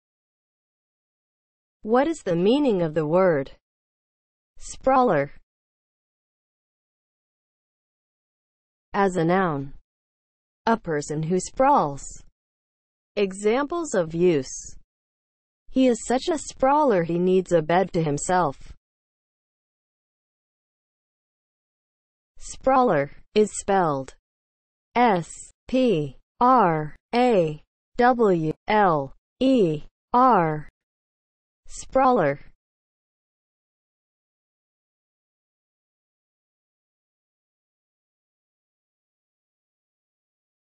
What is the meaning of the word SPRAWLER?
Definition, examples of use and spelling of the word SPRAWLER.

(00:00) Intro
(00:08) As a noun
(00:21) Spelling

As a noun (00:08):
• a person who sprawls

Examples:
• "he is such a sprawler he needs a bed to himself"